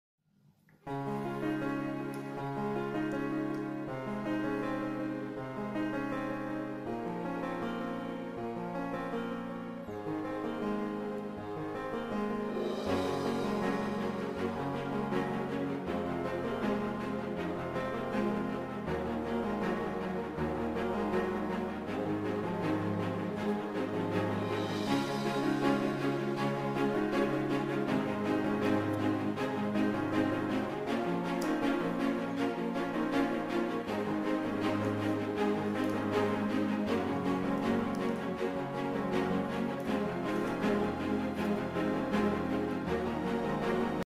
Over 10 000 pearls, Handmade wedding gown by sophie key
1 month to make this custom handmade couture wedding gown